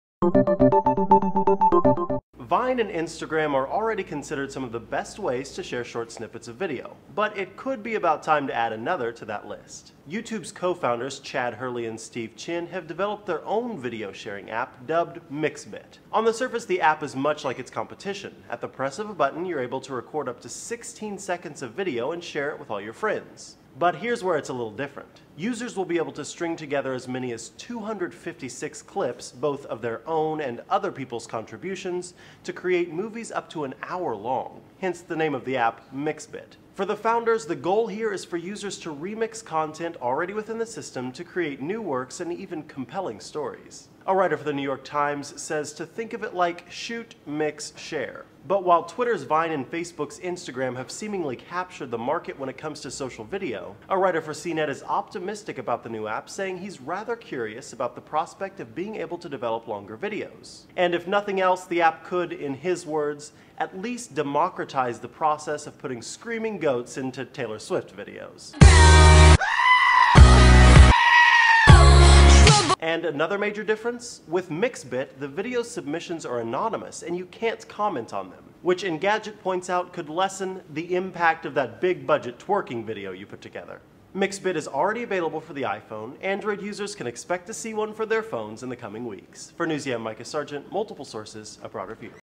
YouTube Co Founders Take On Instagram, Vine With MixBit
유튜브 창업자가 새로 선보인 동영상 서비스인 Mixbit. 바인과 인스타그램과 비슷한 서비스를 제공하는 듯 하면서도 믹싱이 가능하다고 하네요.